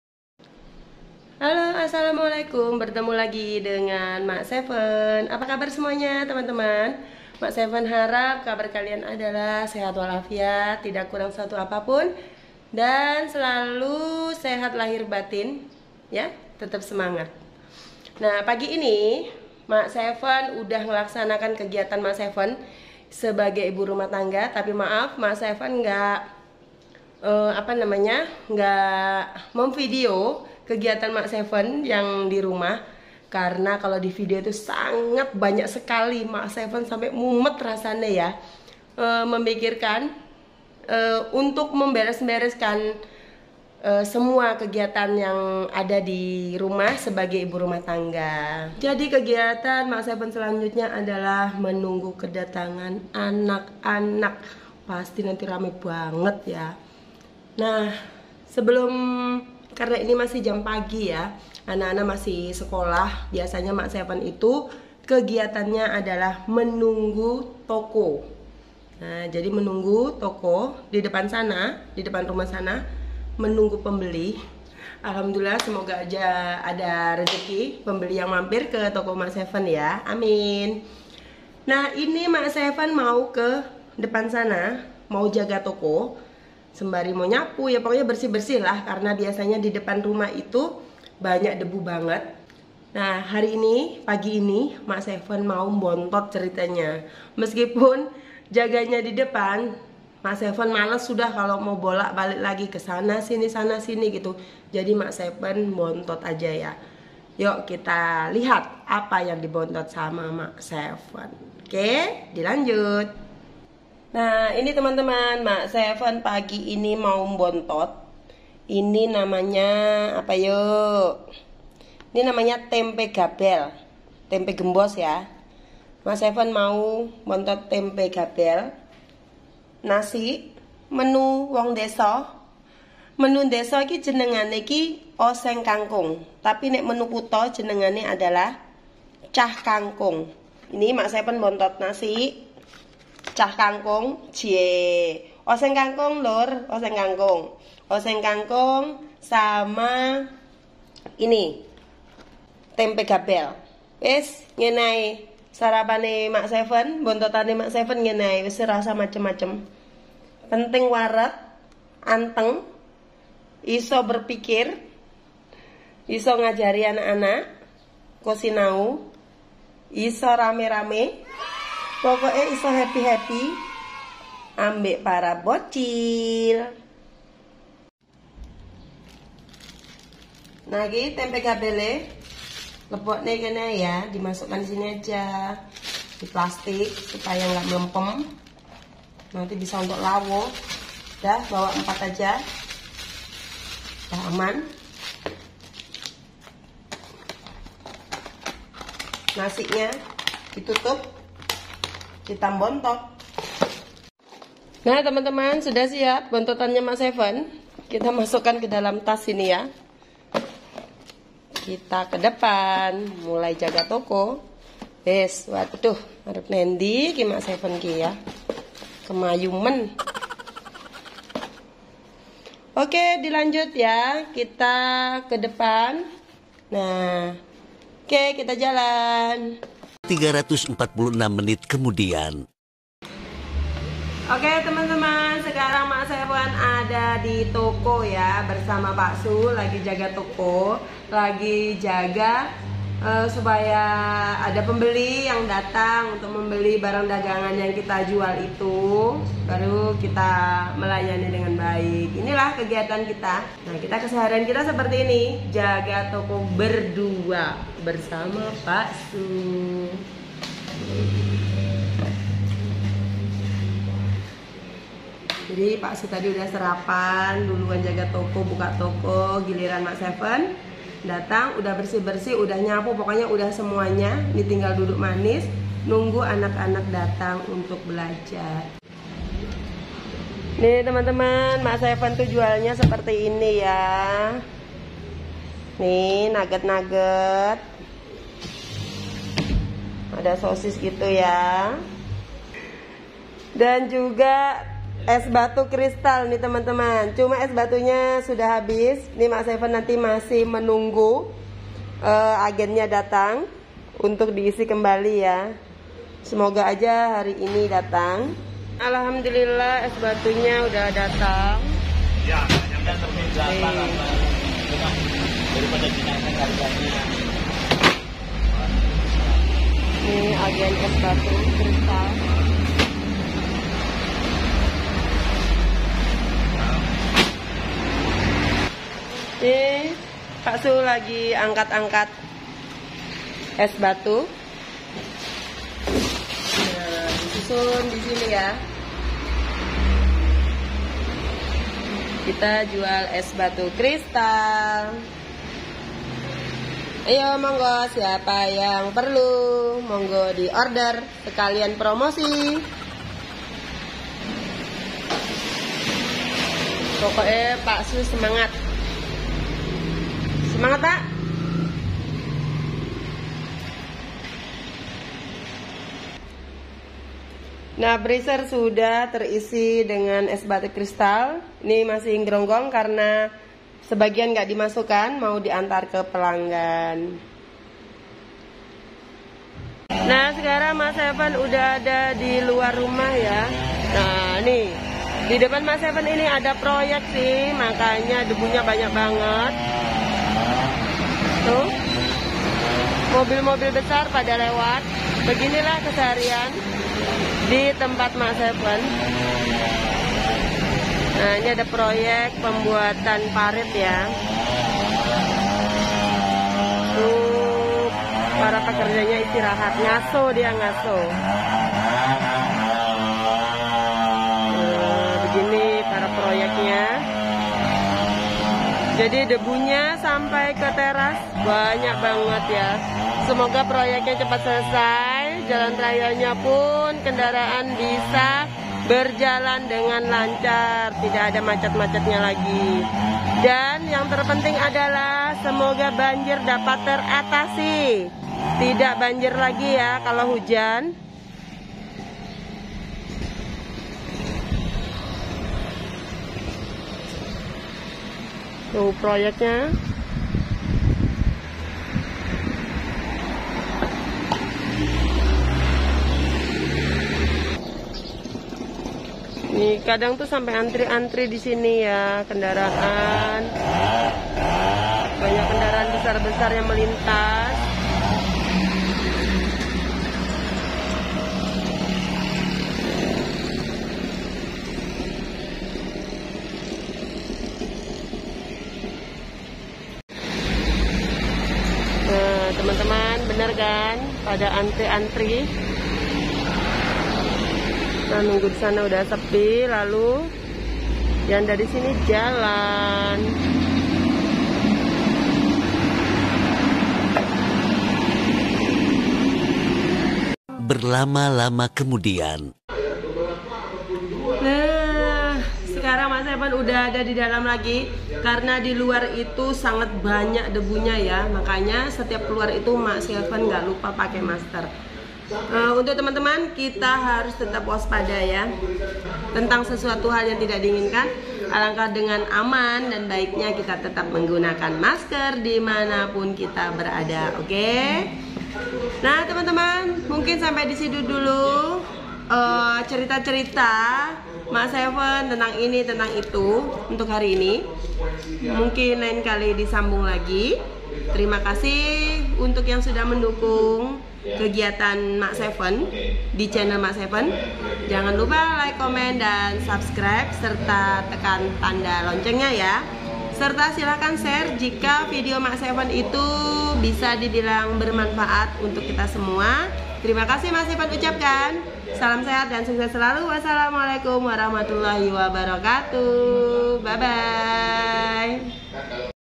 JUALANKU ☝️JAGA TOKONYA BERDUAAN 🤦‼️@MAK SEVEN
Assalamualaikum
Bertemu lagi dengan Mak Seven
Sembari nunggu anak" pulang dr sekolah, Mak Seven kegiatan rutinnya sebelum  mengajar anak" untuk belajar adalah jaga toko berduaan sama PAKSU
Yuuk tonton terus videonya sampai habis yaaaa...full durasi...
Terimakasih atas semua dukungan dari semua teman",🙏
Salam sehat dan sukses selalu